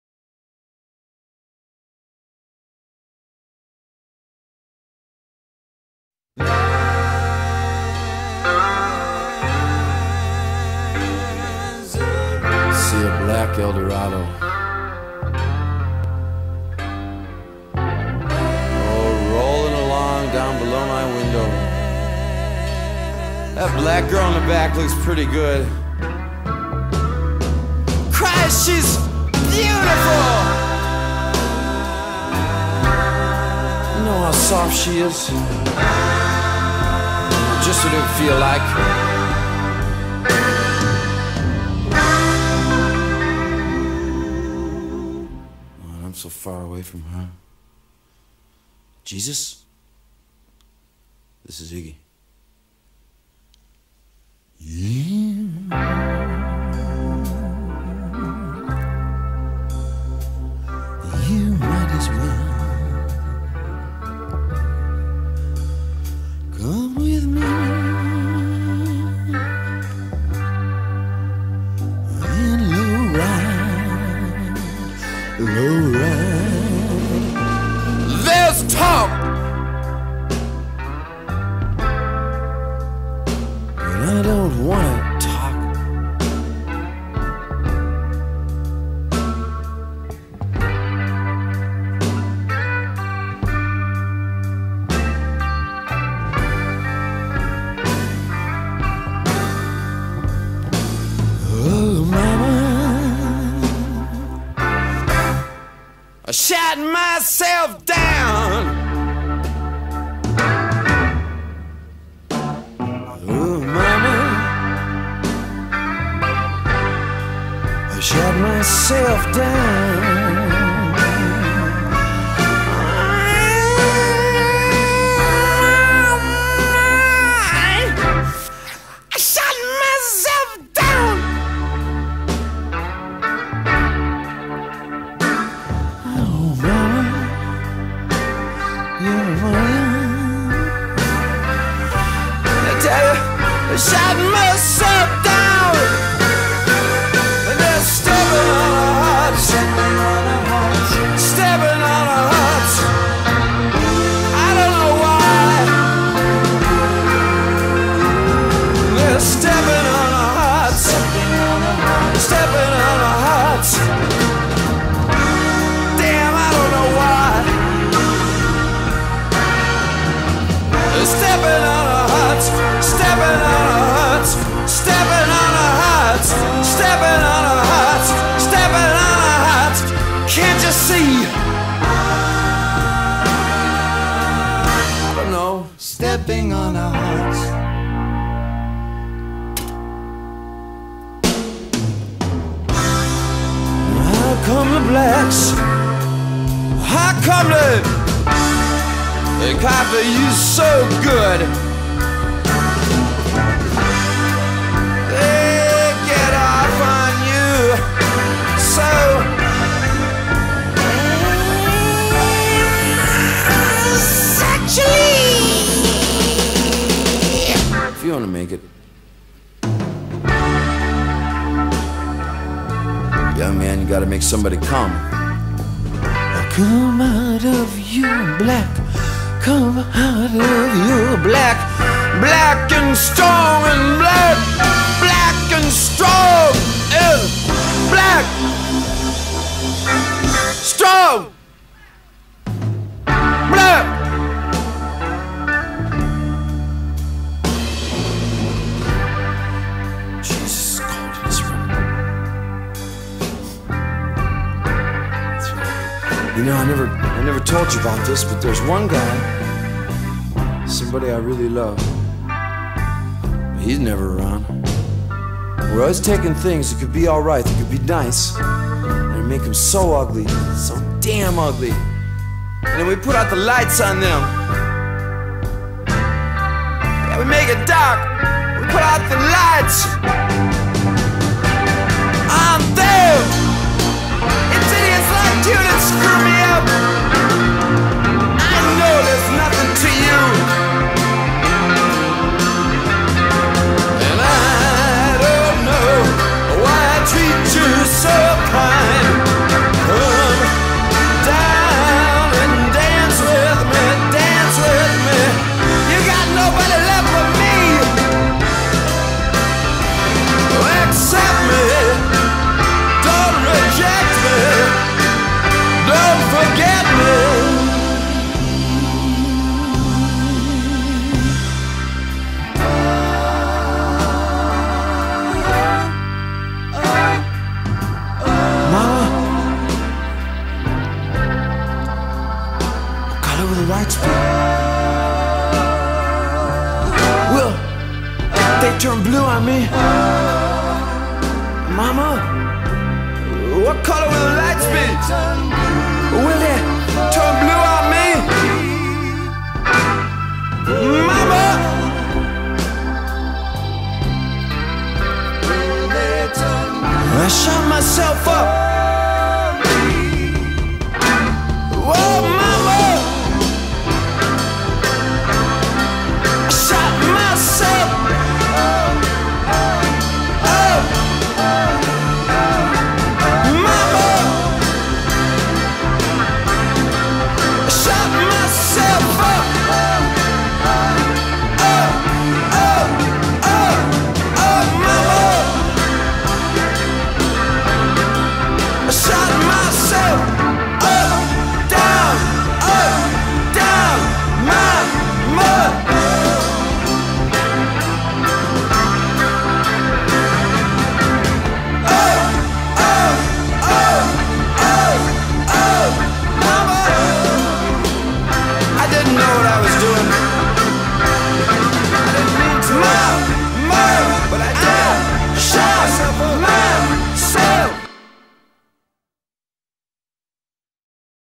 My tribute to Iggy Pop
The inspiration for this tribute stems from the romantic idea that is known as “Gesamtkunstwerk” or “Universal Artwork”. In this particular case photography, video, portraiture and music are used. This tribute has been created out of complete and abundant admiration for Iggy Pop and is for non-commercial and informational purposes with educational content in order to inspire people around the world.